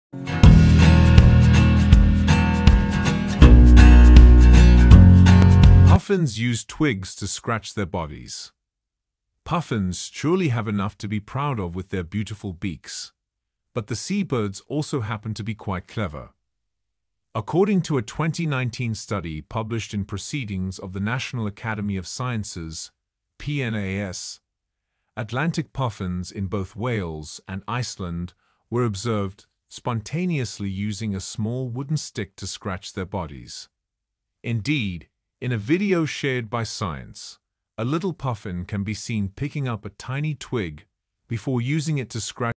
Puffins use twigs to scratch their bodies ||puffins hunt fish || Puffins fact||Pets Paradise..
Puffins use twigs to scratch their bodies ||puffins twings ||Pets Paradise..
Puffins surely have enough to be proud of with their beautiful beaks, but the seabirds also happen to be quite clever. According to a 2020 study published in
 Proceedings of the National Academy of Sciences
Puffins surely have enough to be proud of with their beautiful beaks, but the seabirds also happen to be quite clever. According to a 2019 study published in
amazing knowladge of animals Pets Paradise